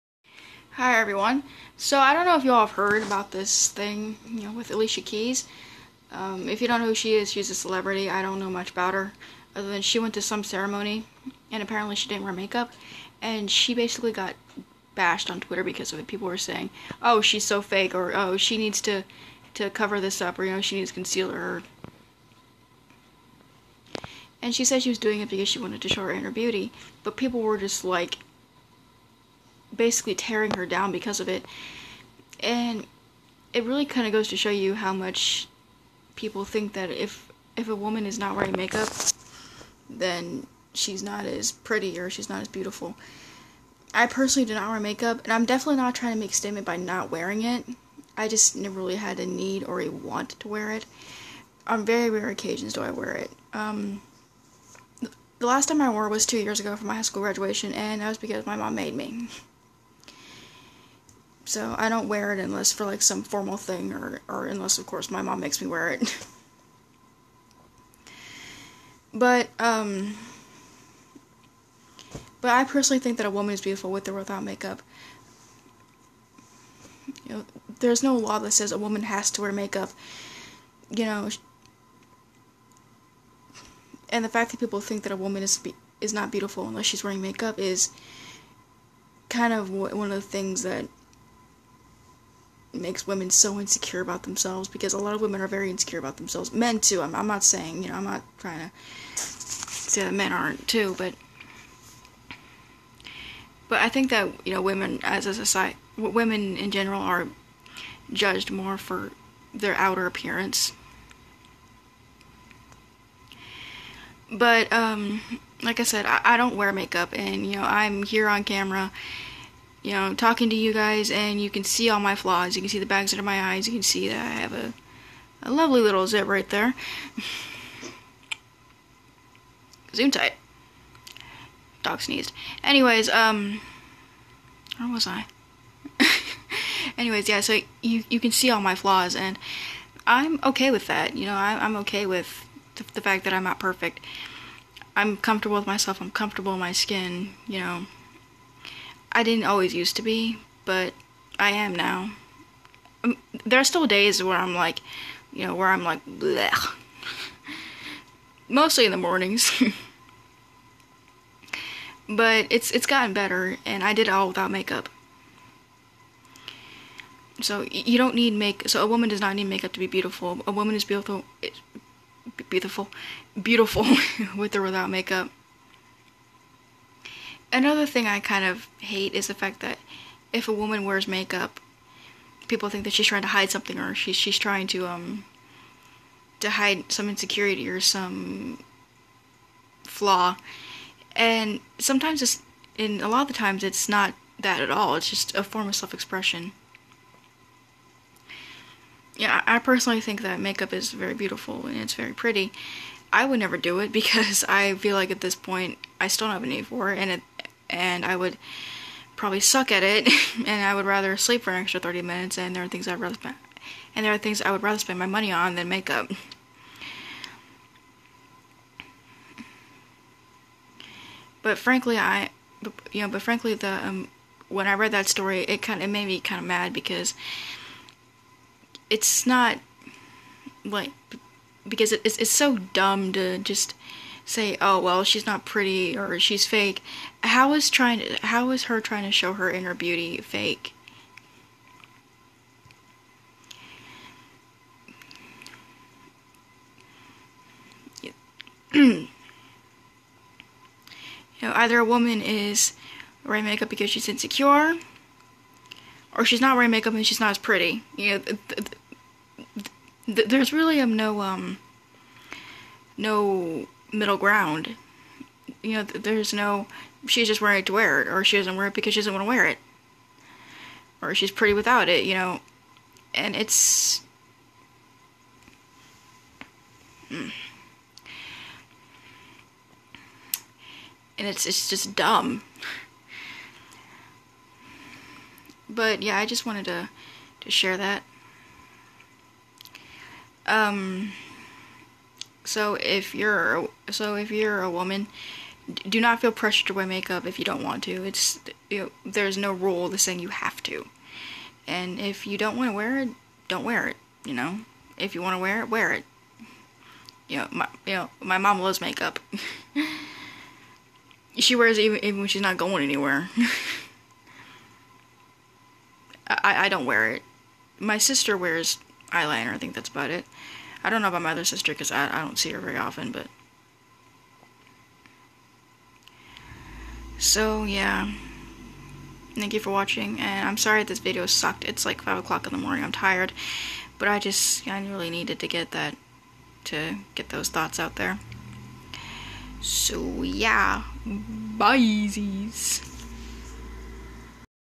My thoughts on make up and how women are judged
Either a woman is judged for wearing makeup or not for wearing makeup. A woman is beautiful with or without makeup.